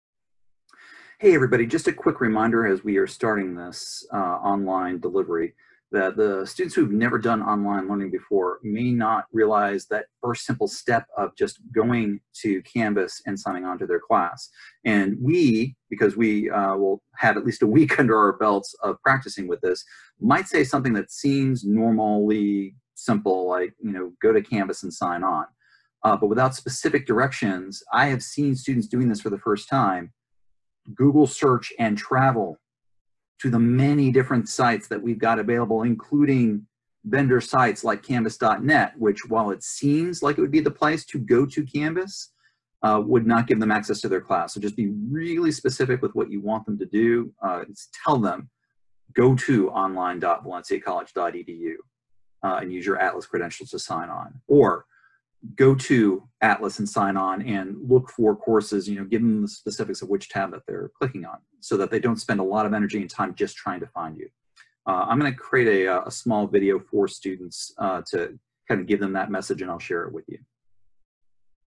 How to invite students to your online class for the first time
We don't know what others don't know. Make your initial instructions simple and exact.